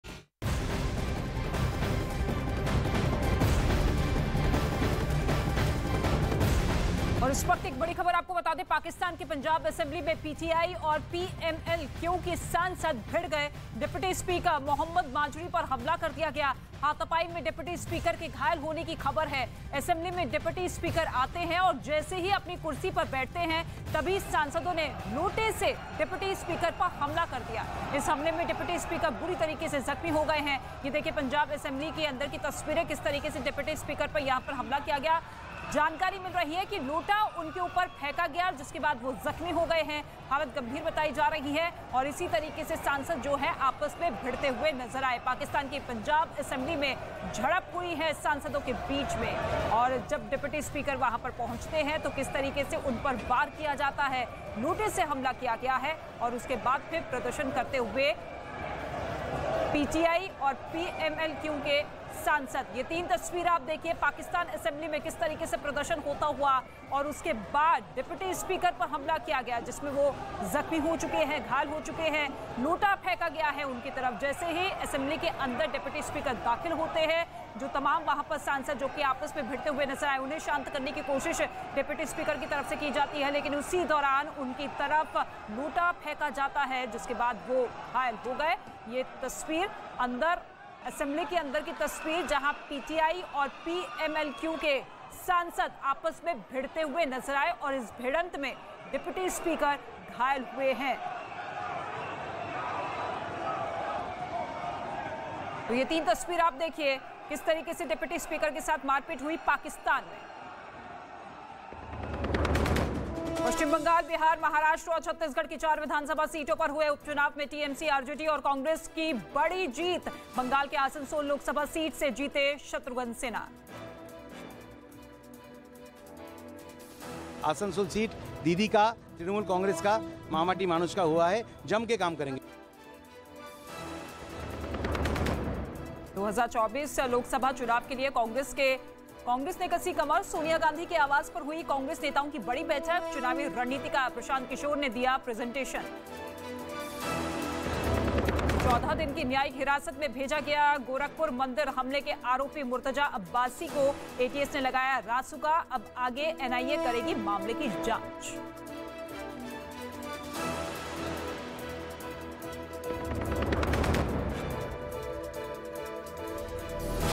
Breaking News: Pakistan की Punjab Assembly बना अखाड़ा, PTI औऱ PML-Q के सांसद भिड़े
Breaking News: पाकिस्तान के पंजाब विधानसभा में शनिवार को जोरदार हंगामा हुआ. जियो न्यूज के मुताबिक इमरान खान की पार्टी पाकिस्तान तहरीक-ए-इंसाफ (PTI) के सदस्यों ने पंजाब विधानसभा के डिप्टी स्पीकर दोस्त मोहम्मद मजारी के साथ कथित तौर पर मारपीट की. वह मुख्यमंत्री का चुनाव करने के लिए बुलाए गए सत्र की अध्यक्षता करने पहुंचे थे.